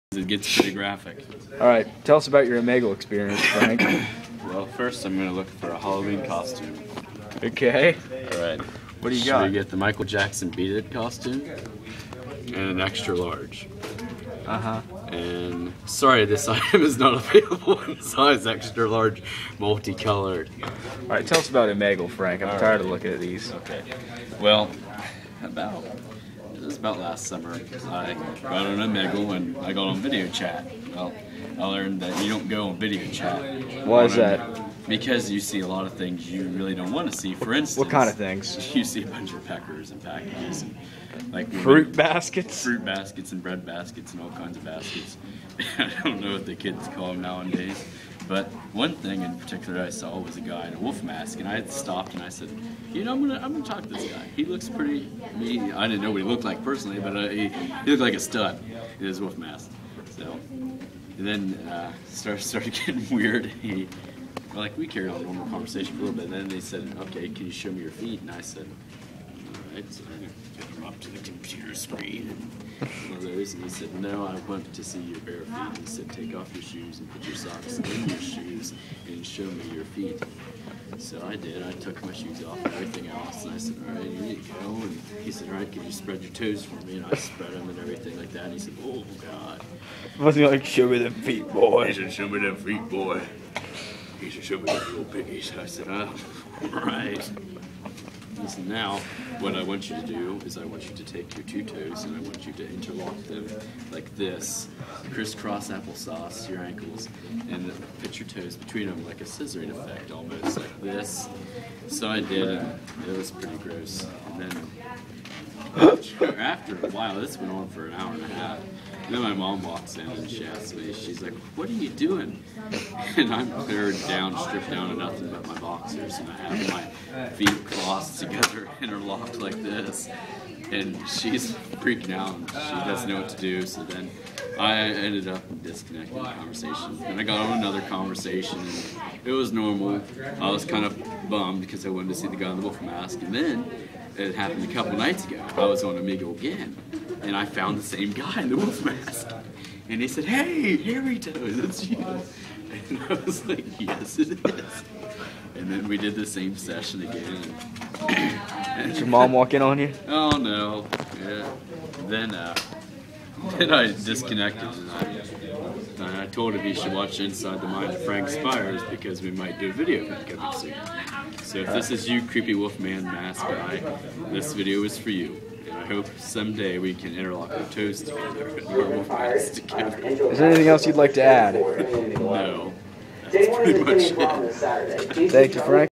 Inside the mind of Frank Spires 3
Frank Spires talks about the things he found on omegle